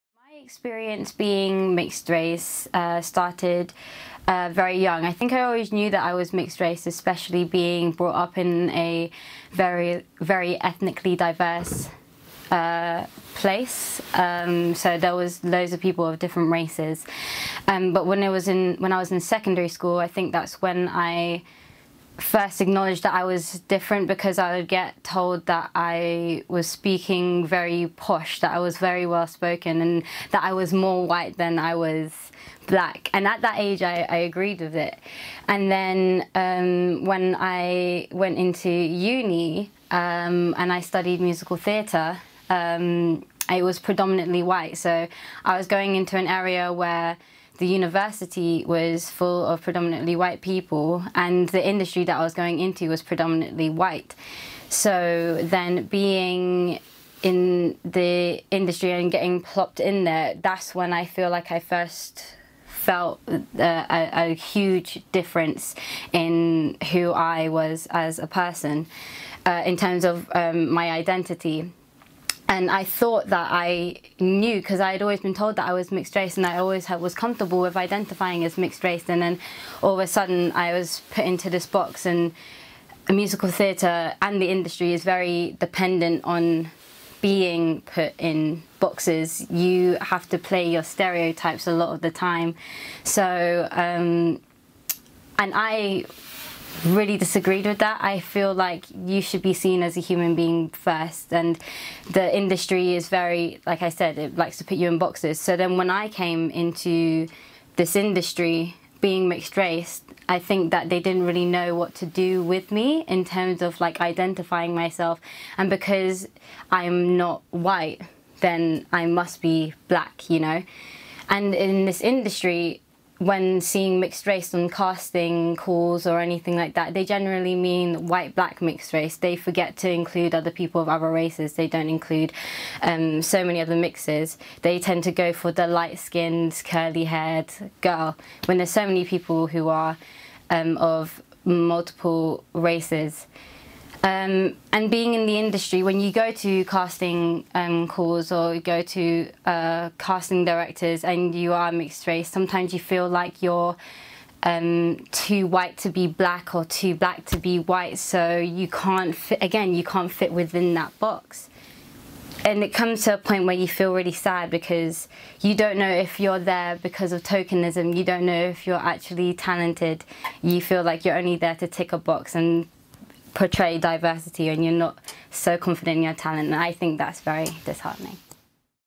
British/Irish | Grenadian
I identify as White/Black mixed race, British, Irish and Grenadian. Both my parents are British born but with parents that have different nationalities. My mother’s parents are Grenadian, and my father has an Irish Father and British Mother. mixedrace @mixedracefaces